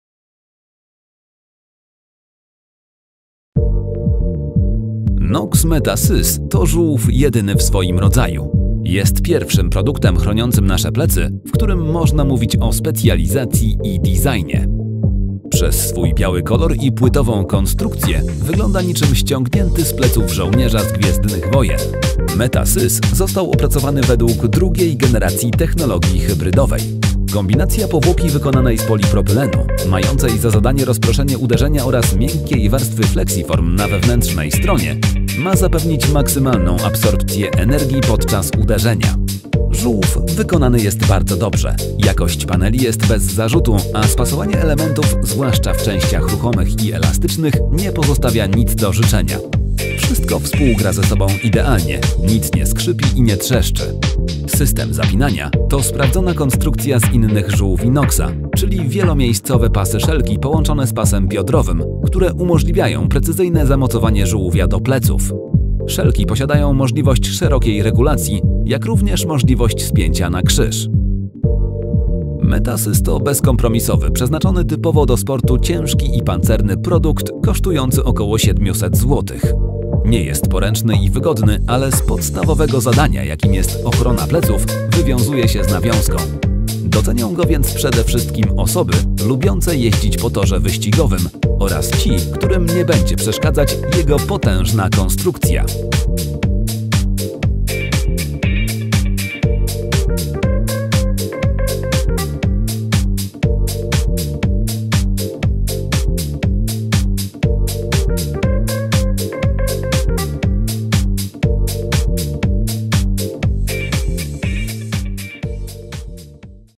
Wideorecenzja: Zółw Knox Meta-Sys
Jakiś czas temu firma wprowadziła na rynek jeden z najlepszych, o ile nie najlepszy, protektor w swojej klasie, żółwia Knox Aegis. Po bardzo dobrym jego przyjęciu przez rynek i użytkowników, firma nie spoczęła na laurach i postanowiła wypuścić na rynek kolejny protektor pleców - Knox Meta-Sys.